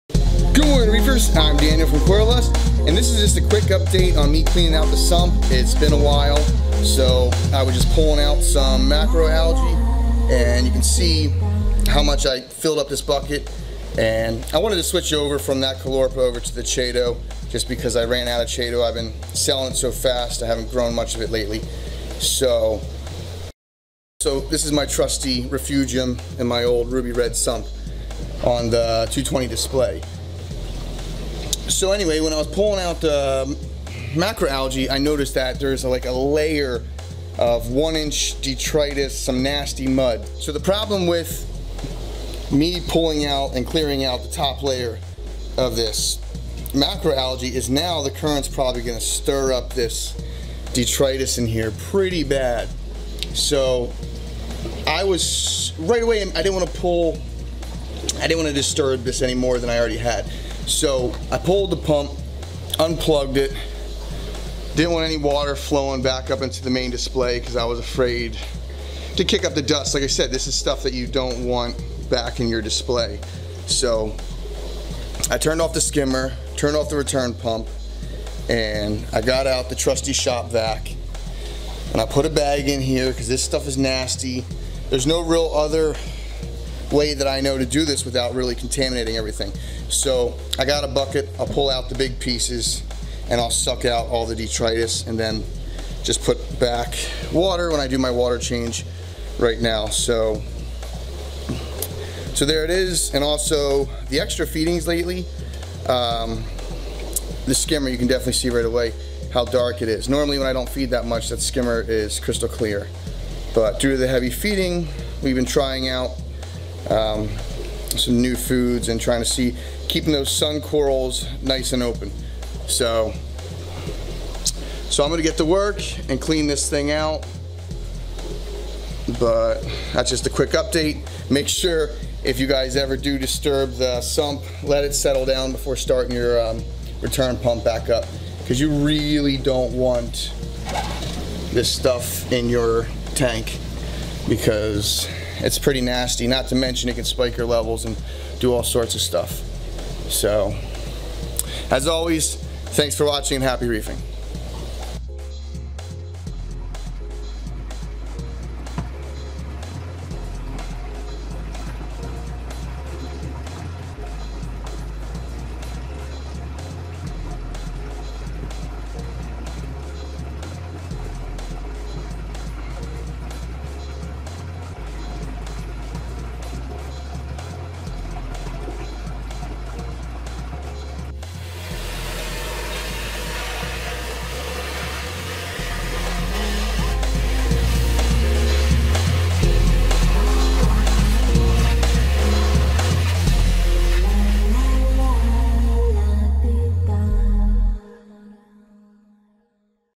Cleaning Detritis out the Refugium
This is a one of the best ways to remove detritis from your aquarium with out disturbing it and contaminating your whole tank.  I recommend regularly scheduled cleaning as part of your tanks maintenance.